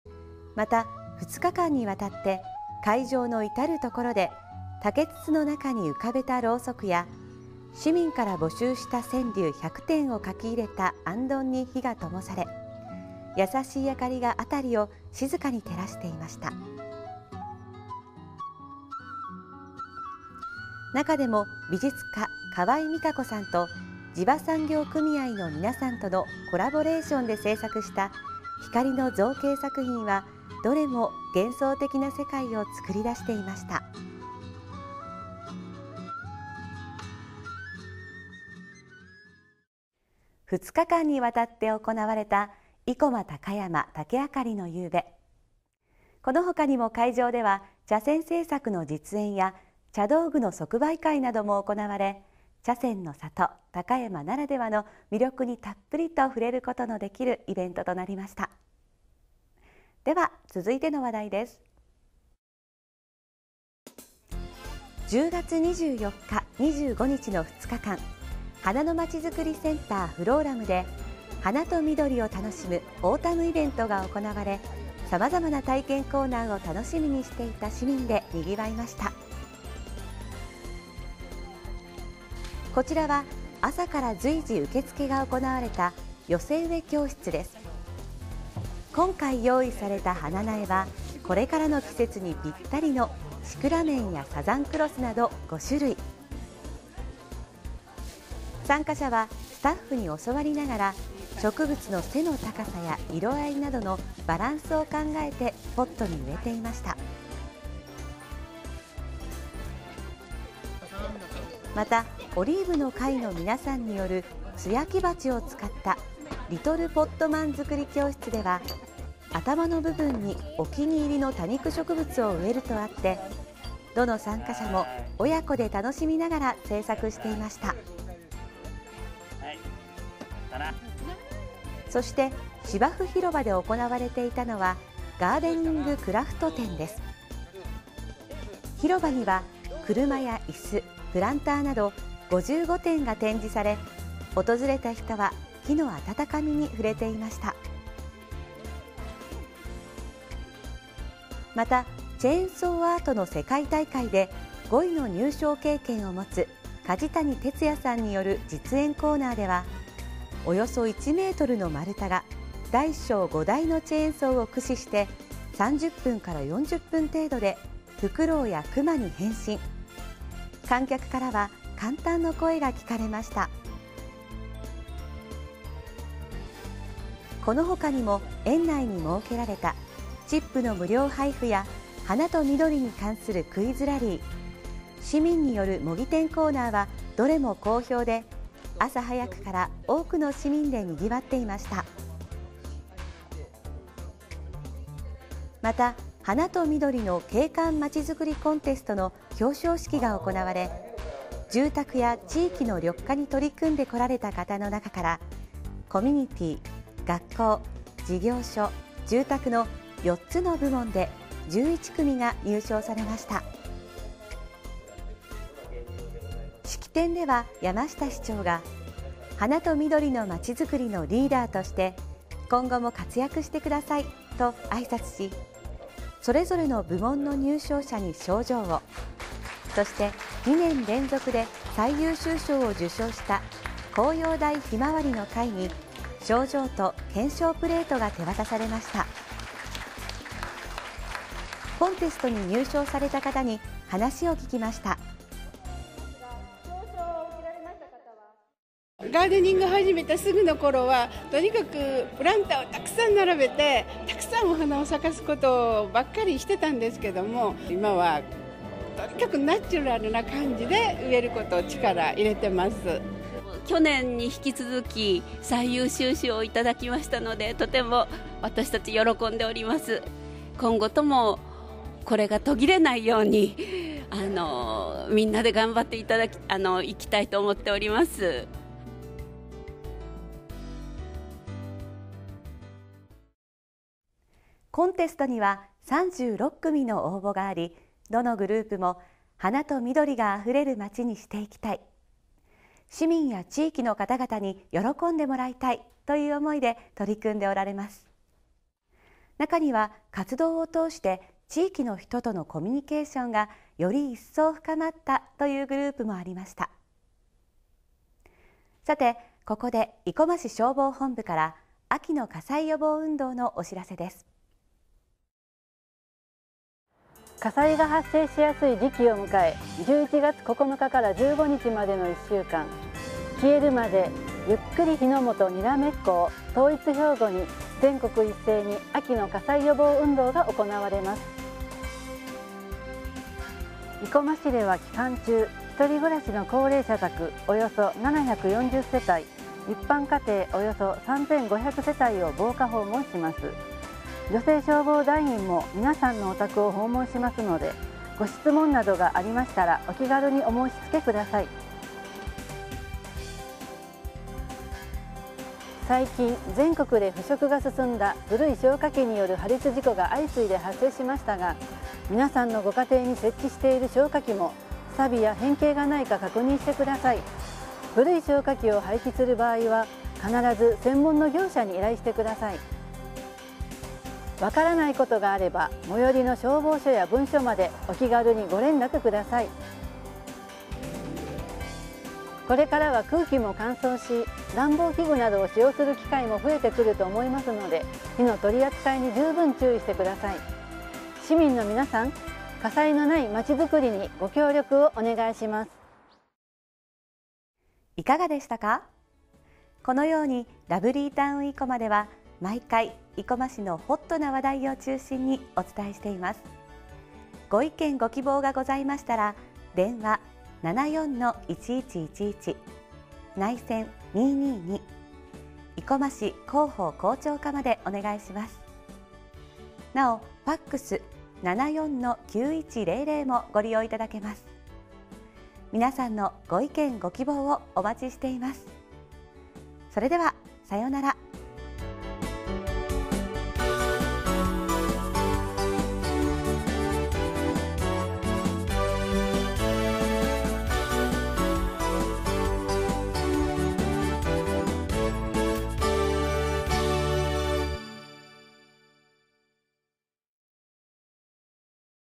生駒市ラブリータウンいこま21年11月1日号(2/2)
平成21年11月1日号
◆市民とともに創る協働のまちづくりシンポジウム（10月18日、コミュニティセンター）
　　生駒市では今年6月に、市民が市政に参画して、よりよいまちづくりのための基本的なルールを定めた「生駒市自治基本条例」を制定しました。この条例を知ってもらうための講演や、山下市長や仲川奈良市長を交えたパネルディスカッションが行われました。
◆幻想的世界　生駒・高山 竹あかりの夕べ（10月23・24日、高山竹林園）
　　高山竹林園で生駒・高山 竹あかりの夕べが開かれ、美術家・川井ミカコさんと地場産業組合の皆さんとのコラボレーションで製作した光の造形作品が幻想的な世界を創り出していました。
◆ふろーらむオータムイベント（10月24日・25日、ふろーらむ）
　　寄せ植え教室やガーデニングクラフト展、チェンソーアートなどさまざまなコーナーで賑わいました。また、花と緑の景観まちづくりコンテストの表彰式が行われ、住宅や地域の緑化に取り組んでこられた方の中から、コミュニティ・学校・事業所・住宅の4つの部門で11組が入賞されました。
◆消防本部から『秋の火災予防運動』のお知らせ